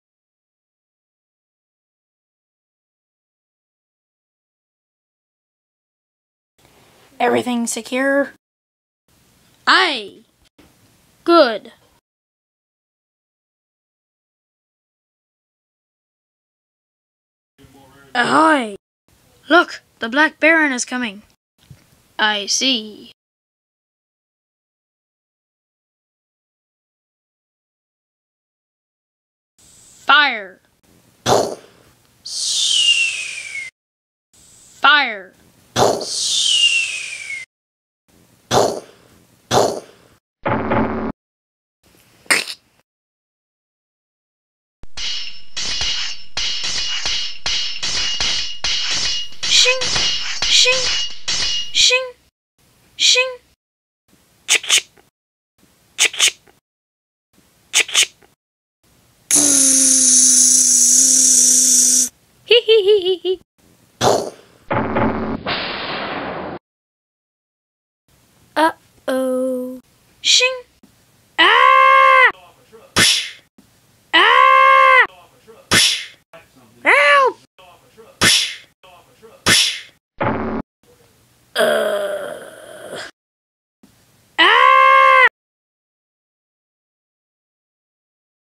Pirates
Two ships duel it out in the block sea